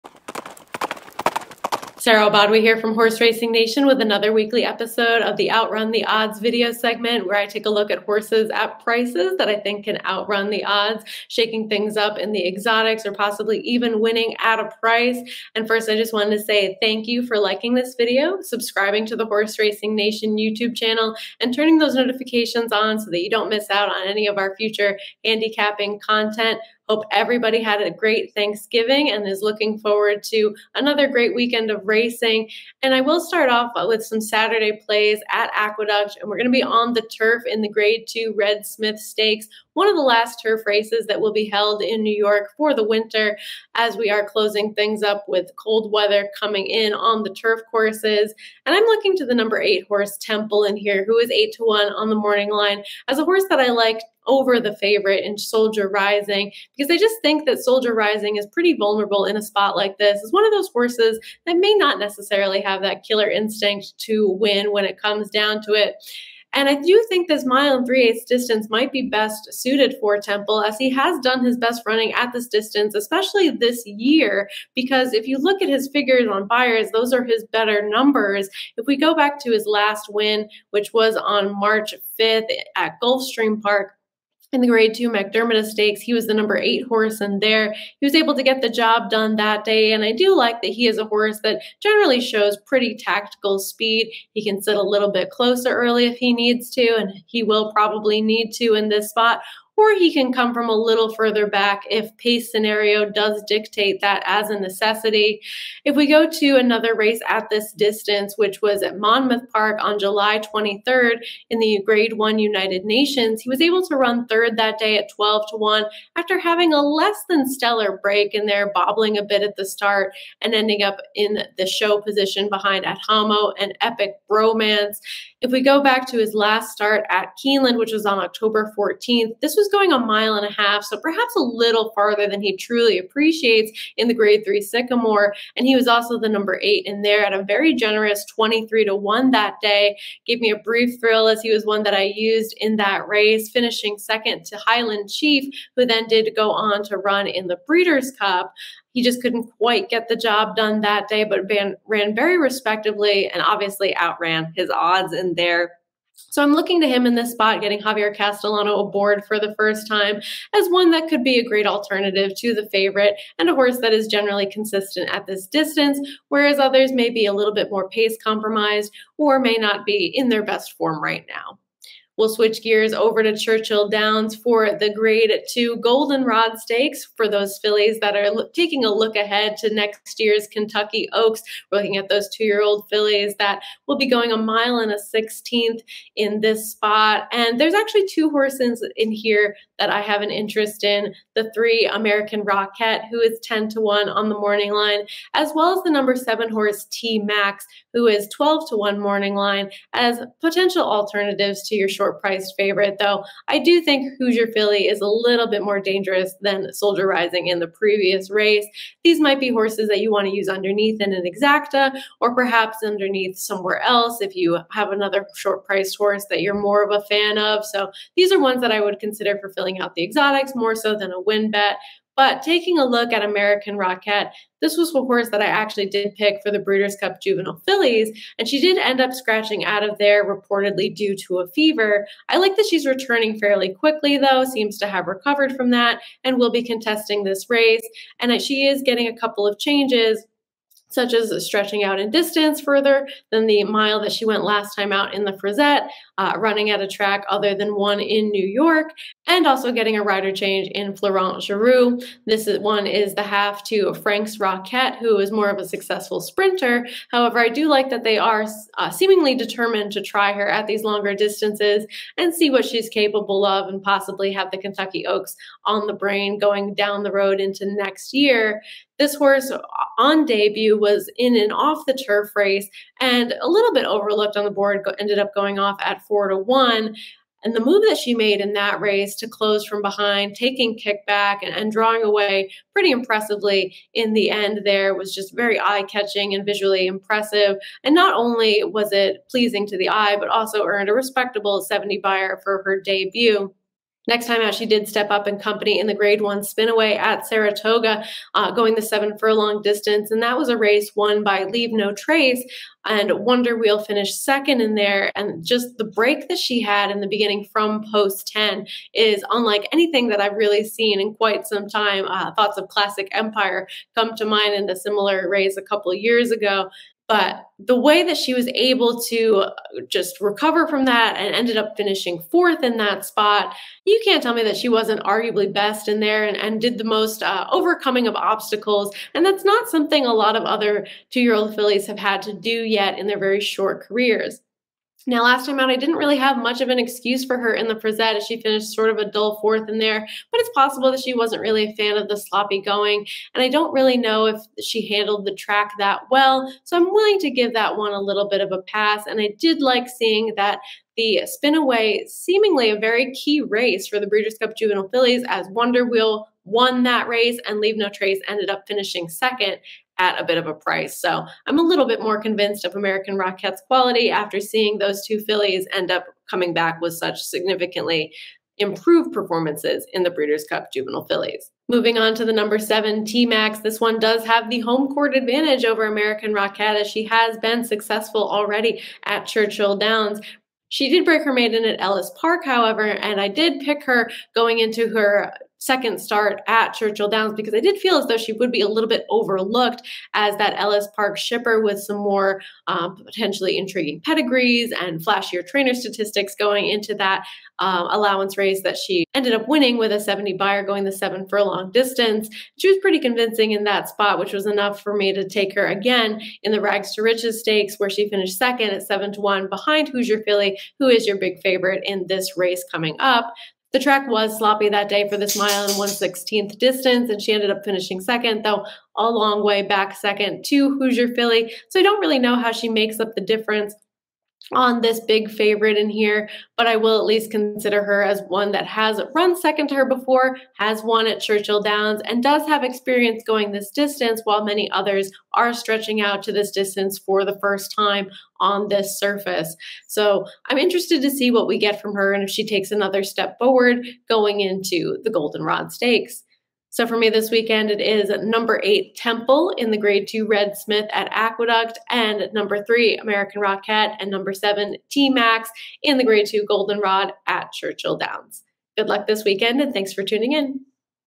Red Smith and Golden Rod Stakes on Outrun the Odds
The 11/25 episode of a segment with Sara Elbadwi on horses that should 'outrun the odds' in upcoming weekend races. This week's races include the Grade 2 Red Smith at Aqueduct and Grade 2 Golden Rod at Churchill Downs on Saturday, 11/26.